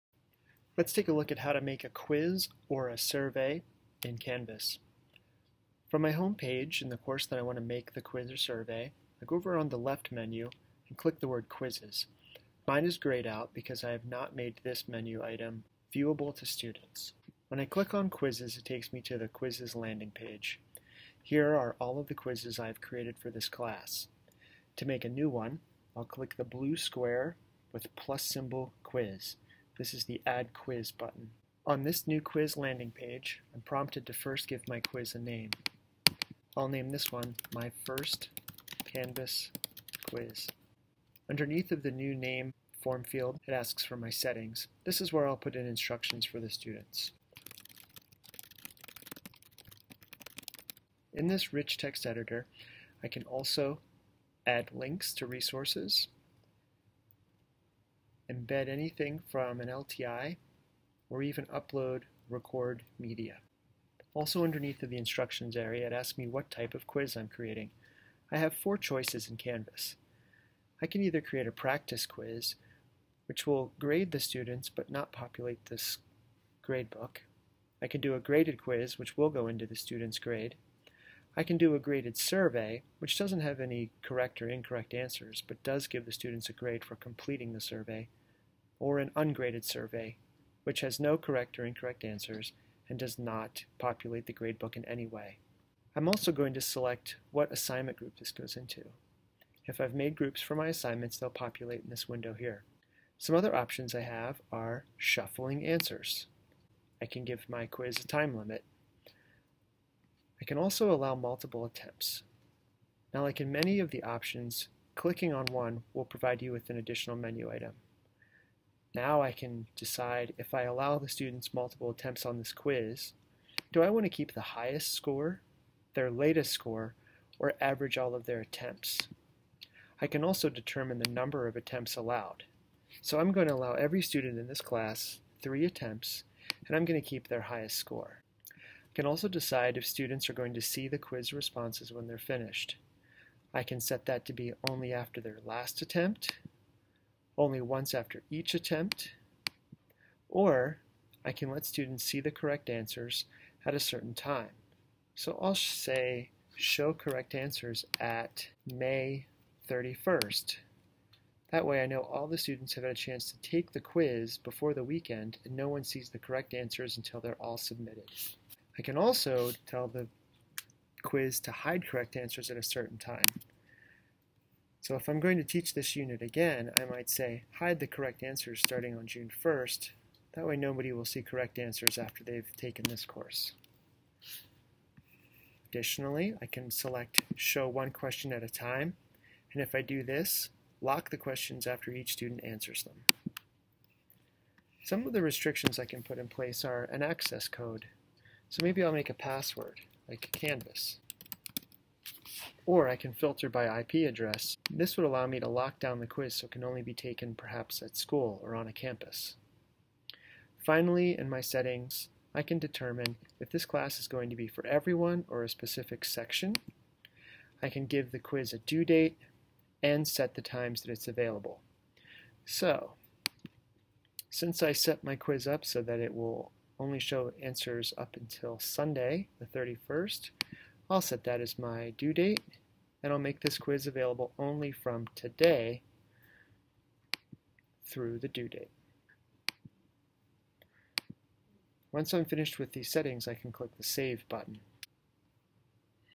Creating A Canvas Quiz Part 1
In this video tutorial I'll show you how to make a new quiz and adjust the quiz settings before adding questions.